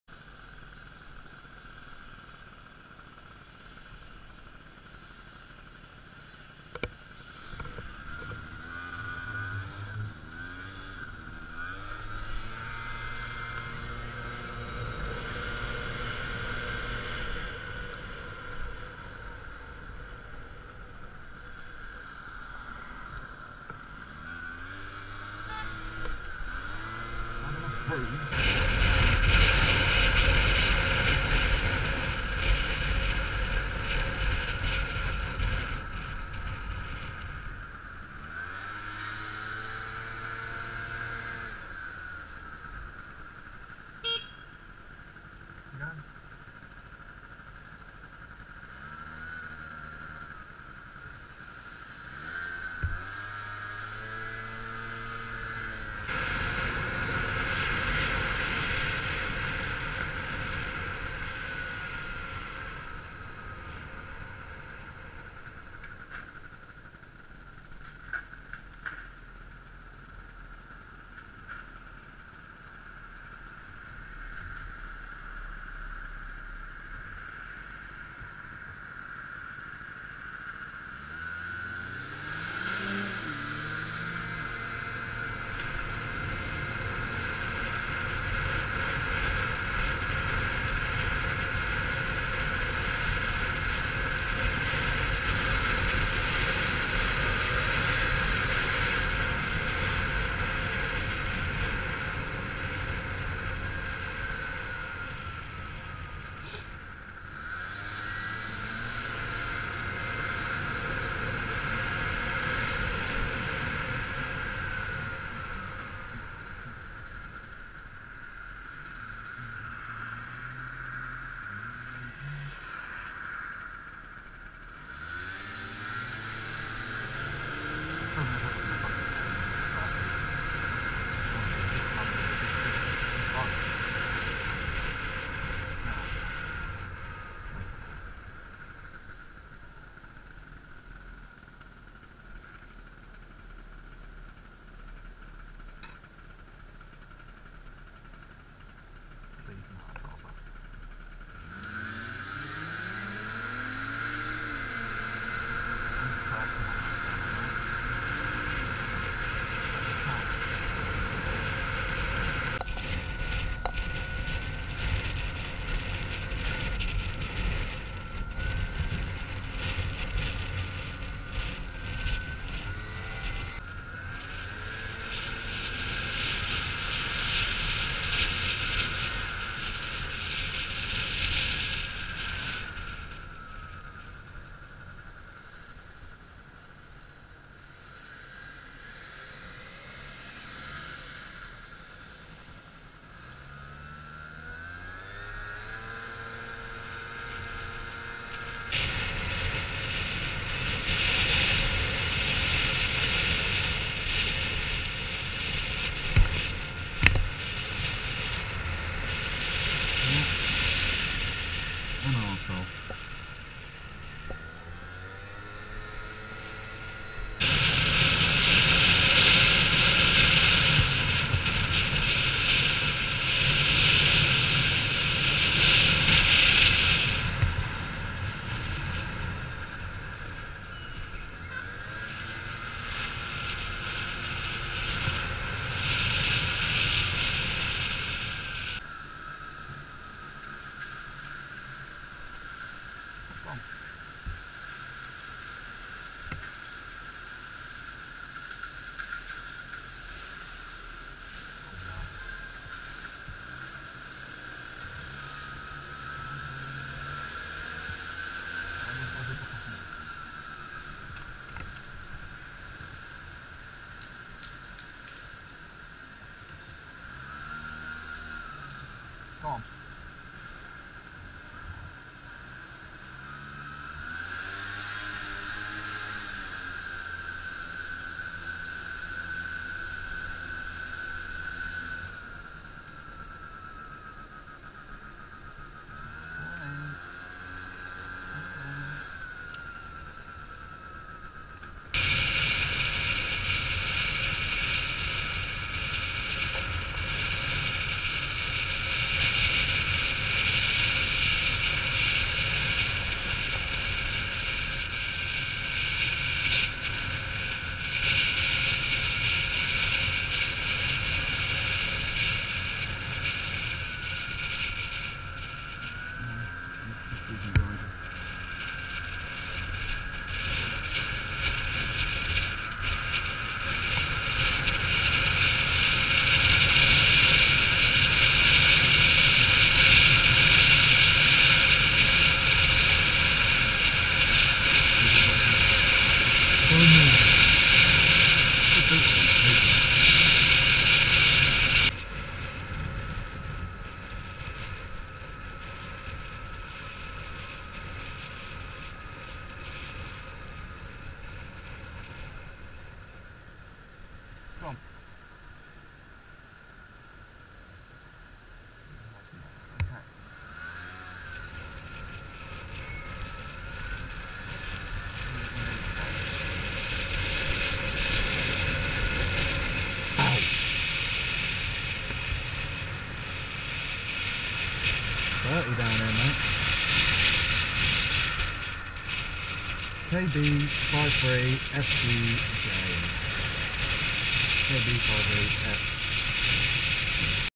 Bad driving and woopsies in northampton
A compilation of bad driving in Northampton.